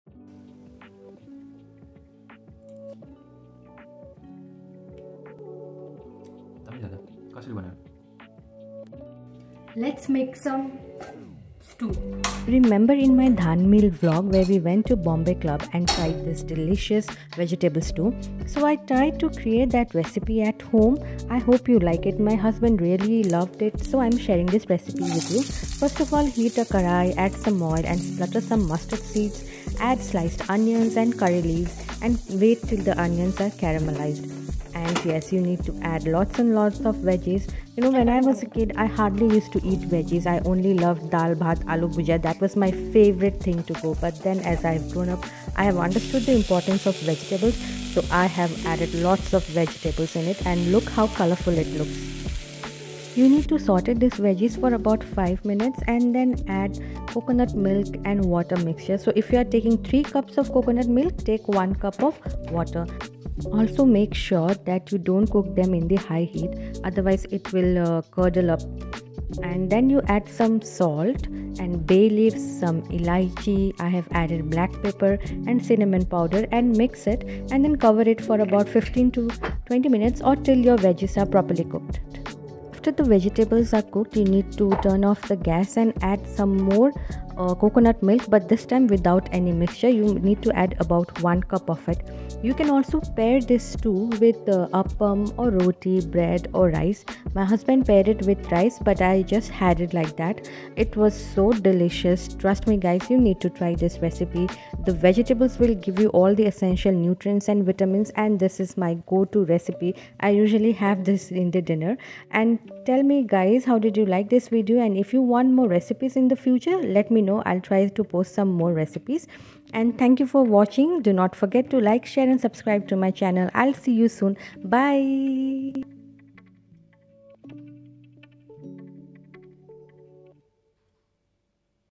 Vegetable Stew Recipe 🚨
I tried to recreate the vegetable stew that we had in the Bombay club. I hope you like the recipe.

Music from Uppbeat (free for Creators!)